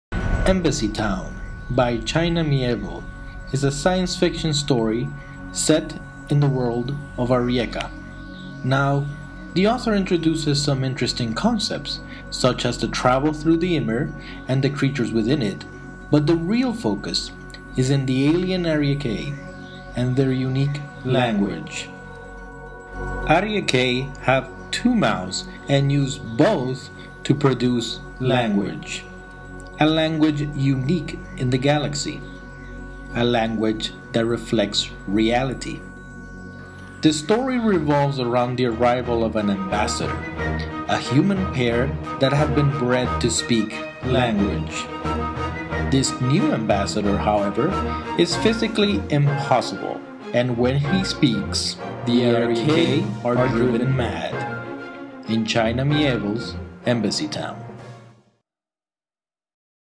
Embassytown by China Mieville
A quick, 1-minute summary/review of China Mieville's Embassytown.